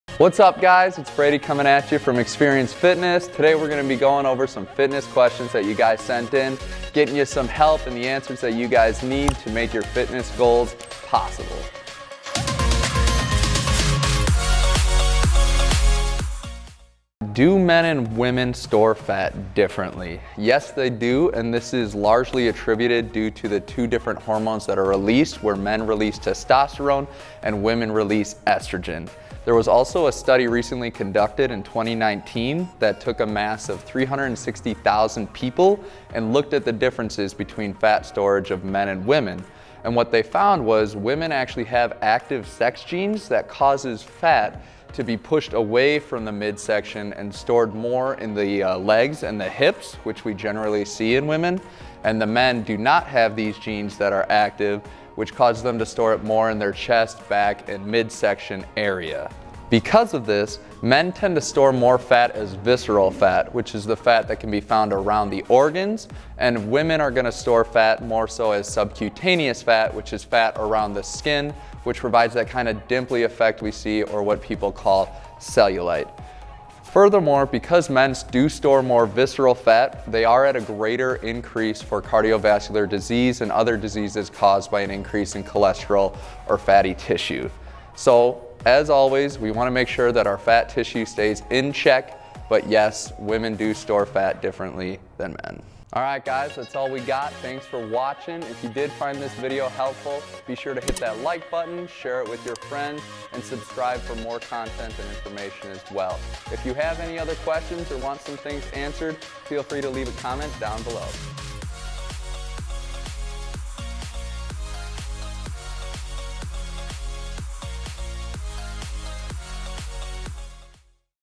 Do Men and Women Store Fat Differently? | Xperience Fitness
Xperience Fitness personal trainer Brady explains why men and women store fat differently. He mentions the main difference between storing fat between the genders are their sex hormones (estrogen and testosterone). Women have an active sex gene that pushes fat away from the midsection towards the hips and thighs. Men don’t hold this gene so their fat is stored in their chest, back, and midsection.
He also mentions that since men store more visceral fat, they are at a greater risk for cardiovascular disease. Check it out to find out more. 😎